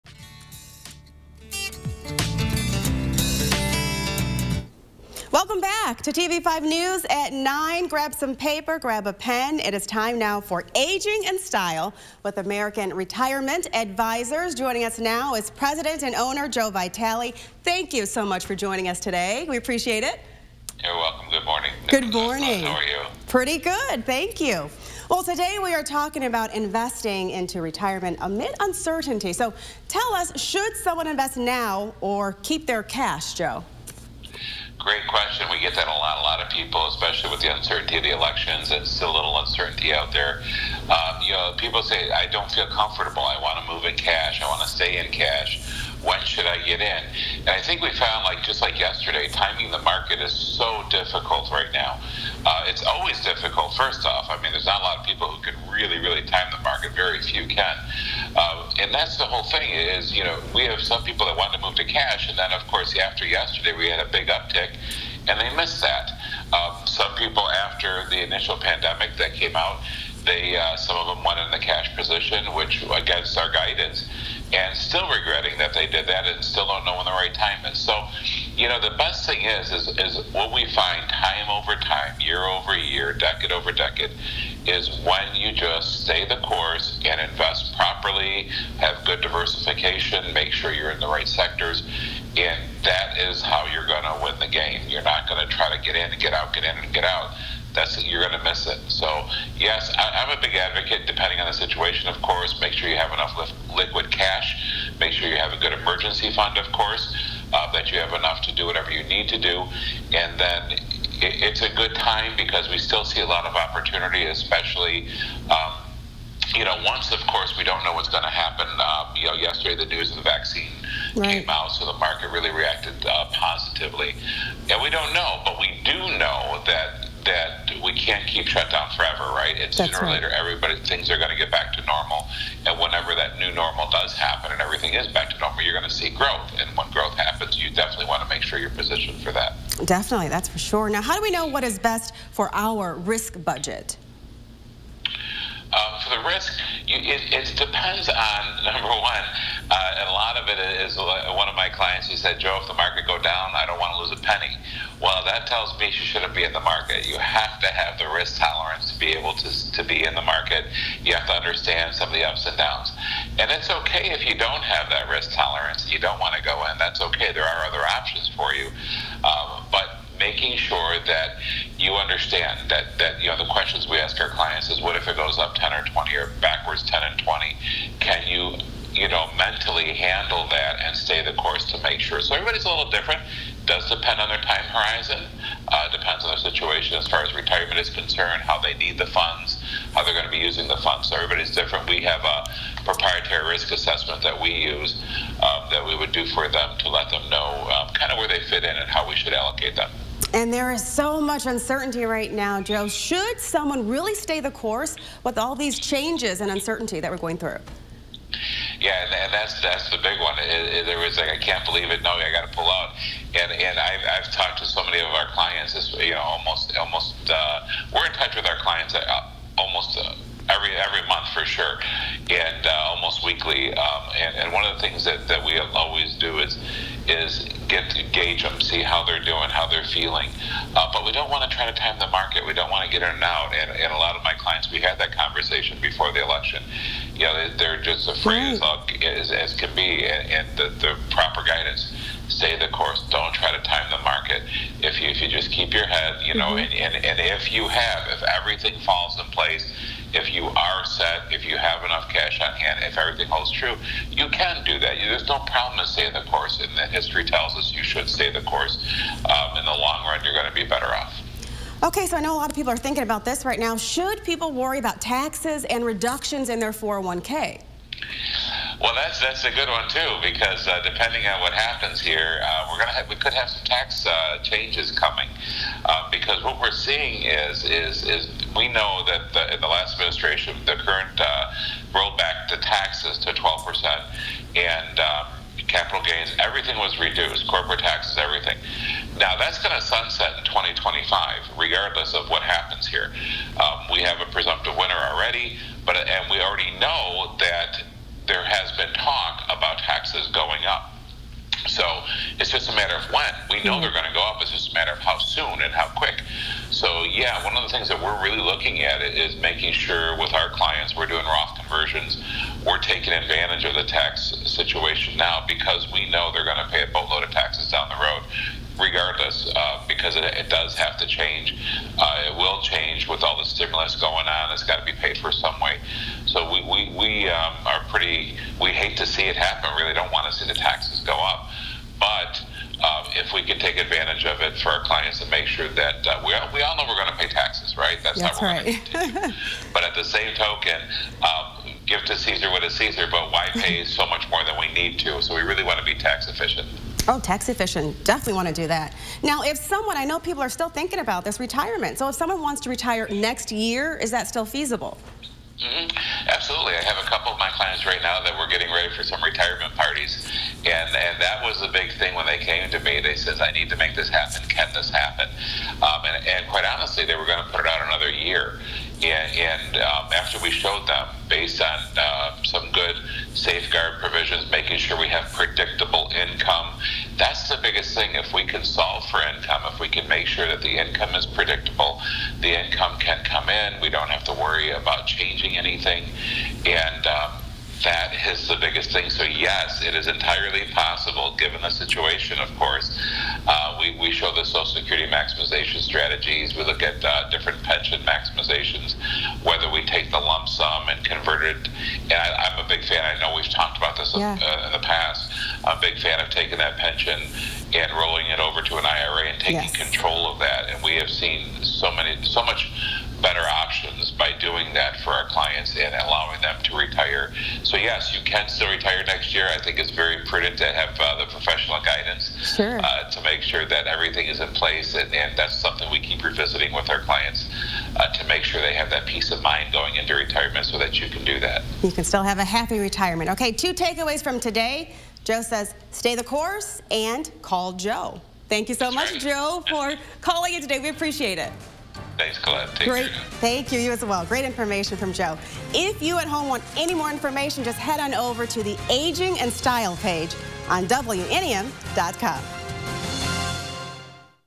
Aging in Style: Investing in retirement
American Retirement Advisors is joining us to talk about investing during a time of uncertainty.